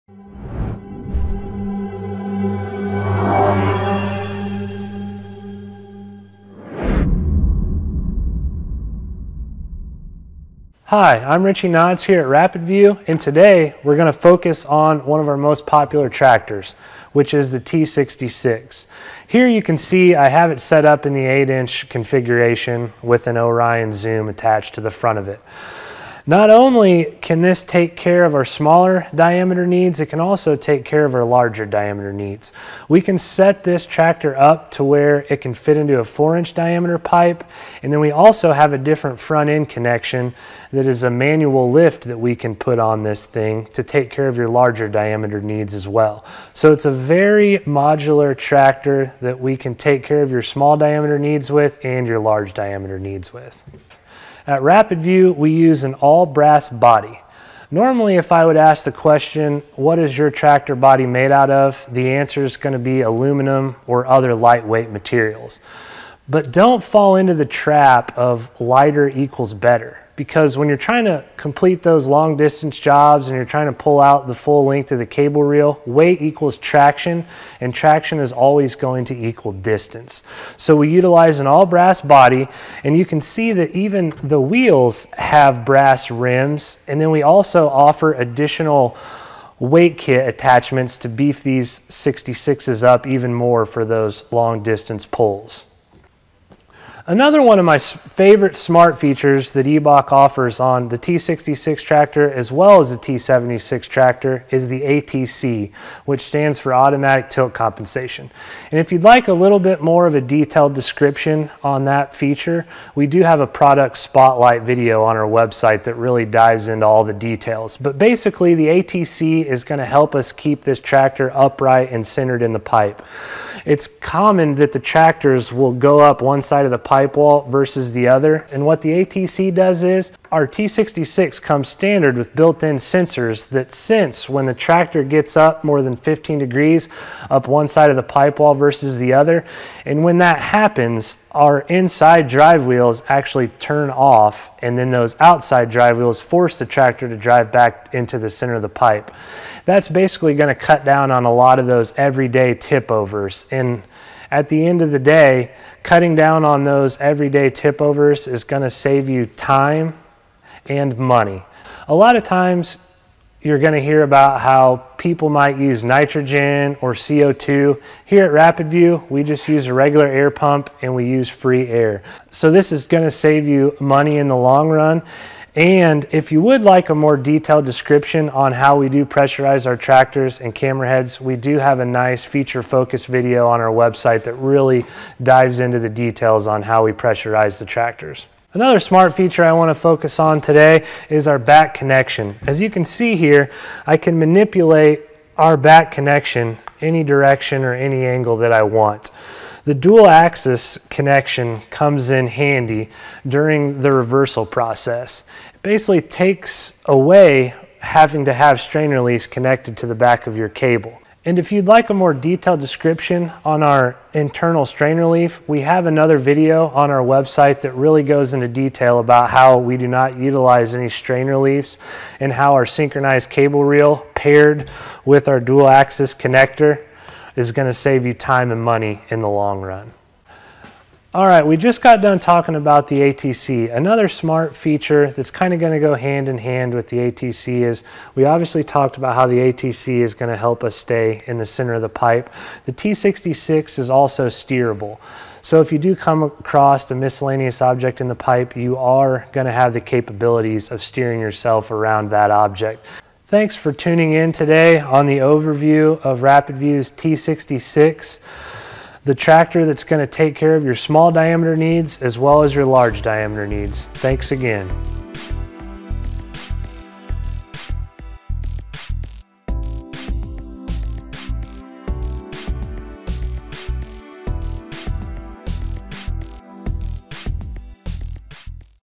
T66 Camera Tractor Overview: Small, Powerful Sewer Inspection Crawler
Learn about IBAK's T66 Camera Tractor, a small yet powerful sewer inspection crawler that boasts a heavier chassis for added traction, smaller external dimensions for ease of use, and greater pulling strength than its predecessor. Ideal for inspecting pipelines 4" (100mm) and up.